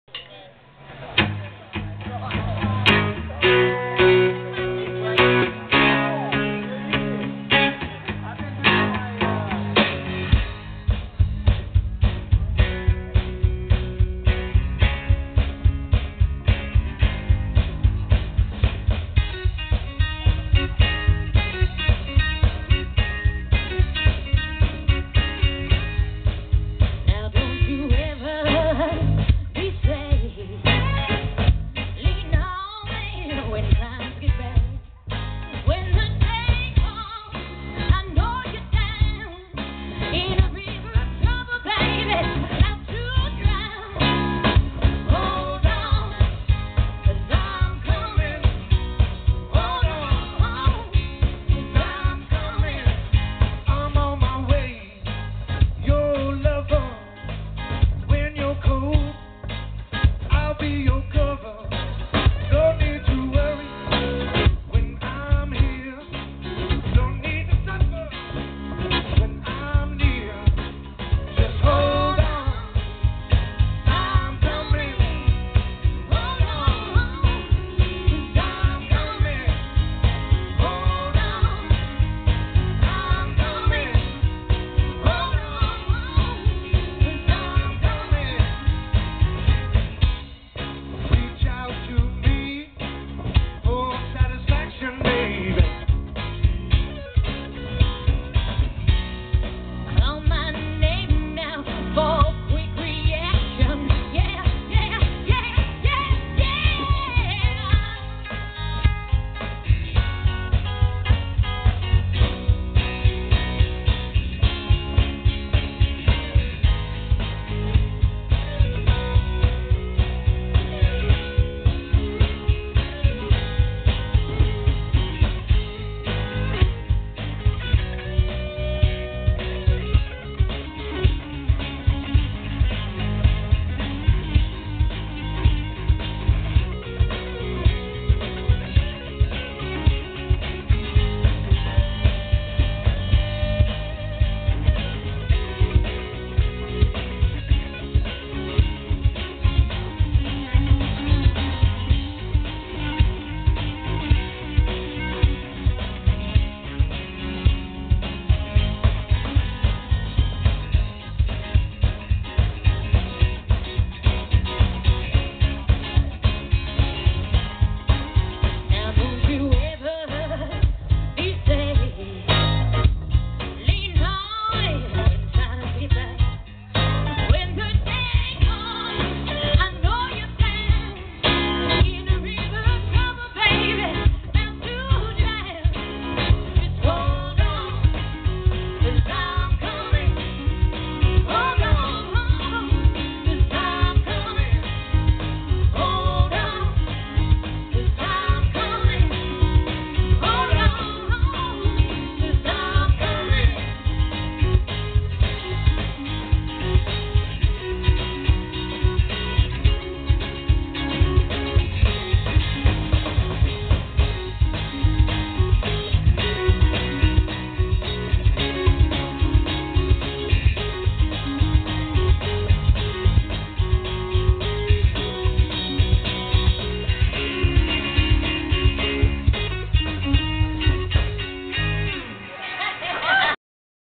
Shawn Starski
Live @ The Dry Riders Party In Williansburg Ohio @ Mr Willy's House